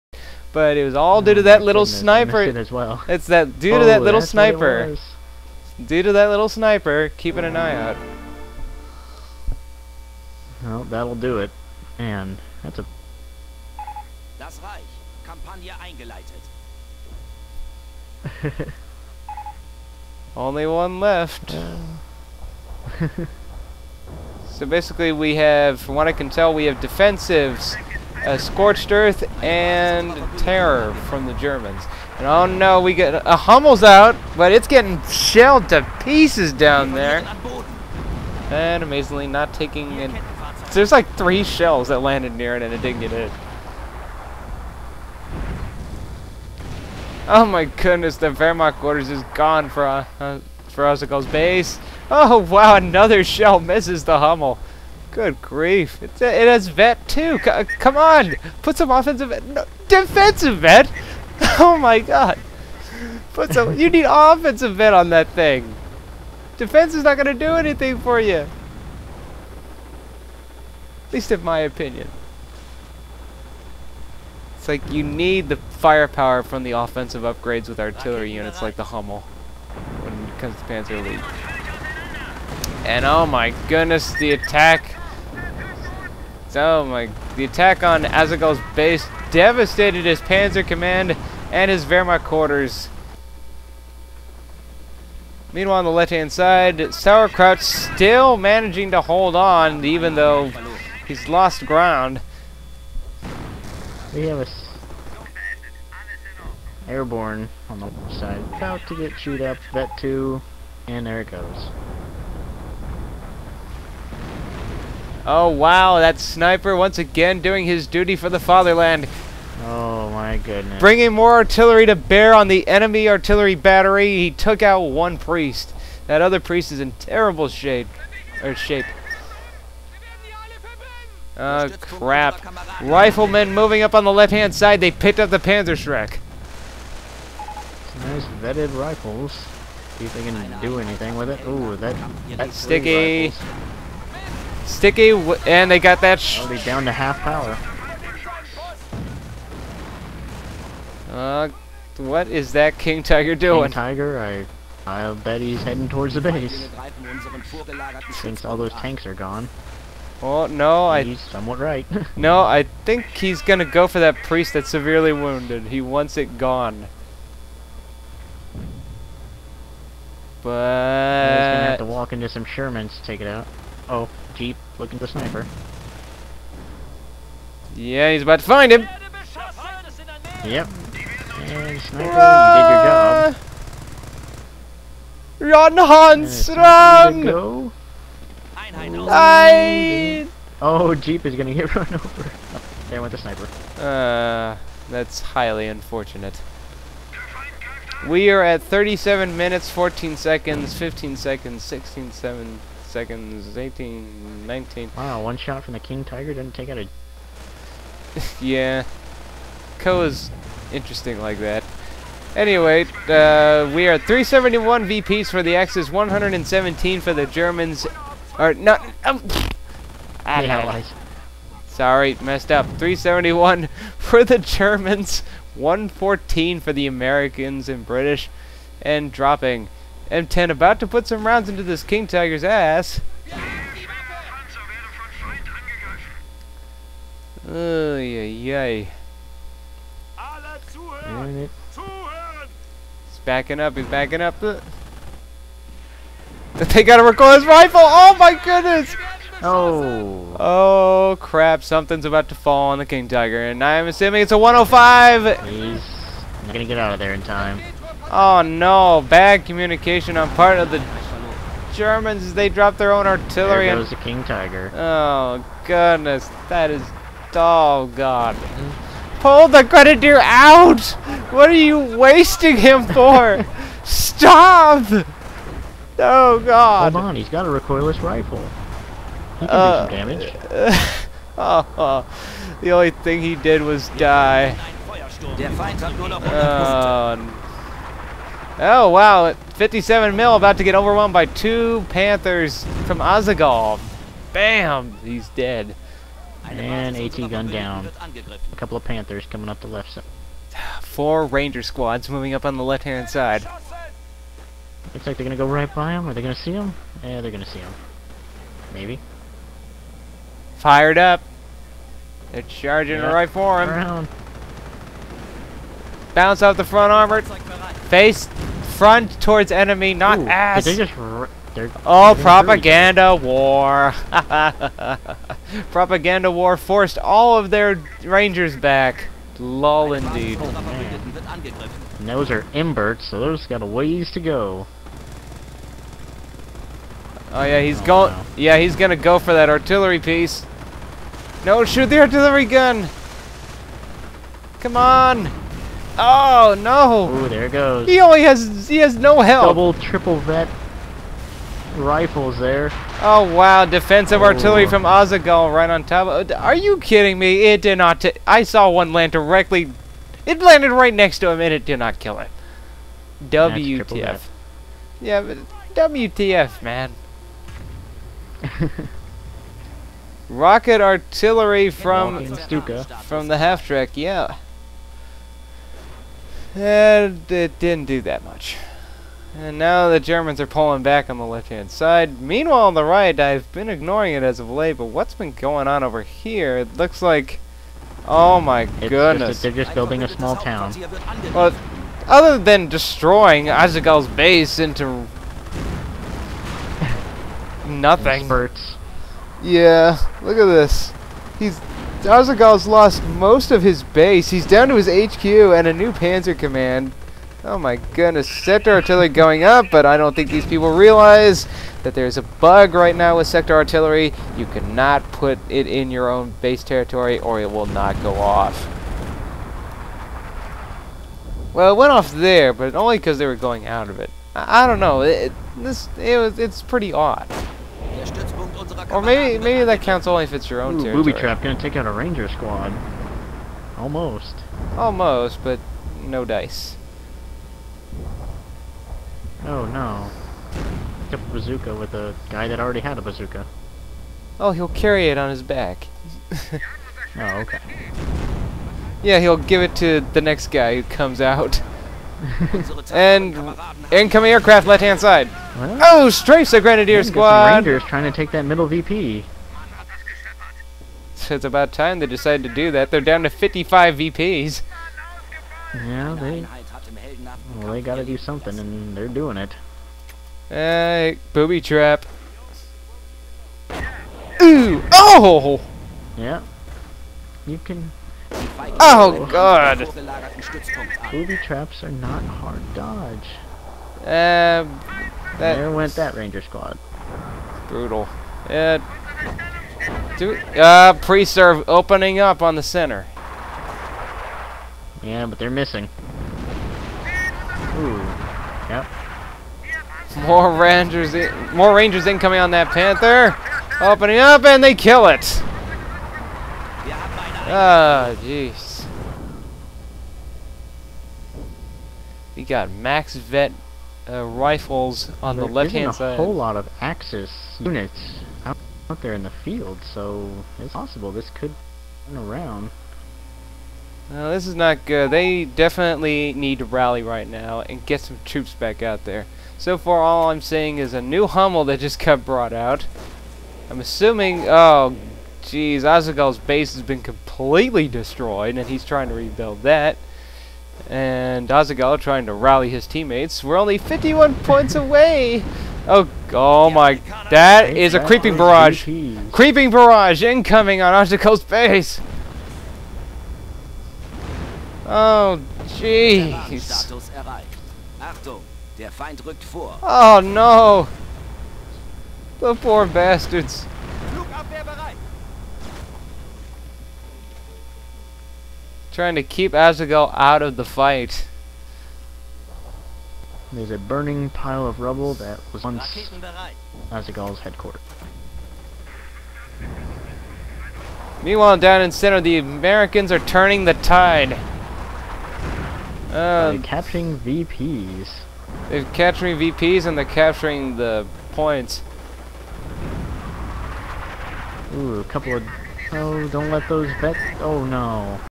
OSC Vidcast #27 part5
After a lot of Left 4 Dead I get back to business. Though unfortunately alot of people were out for this 3v3 I was able to get a new comer to the GameReplays.org shoutcaster. His callsign is Cindex and he will be accompanying me on this excursion to Hill 331!

More bloody madness with all 4 factions involved and alot of destruction is sure to come. Can the Germans with their combined might win the day? Or will the British Artillery with American bravery undermine their efforts? Watch and find out!

[Allied Team]
ITCOIBellicosity (Airborne)
Mbert (Infantry)
Finity0 (Royal Artillery)

[Axis Team]
Azaghall (Defensive)
gooderthanyou (Scorched Earth)
SourSauerKraut (Terror)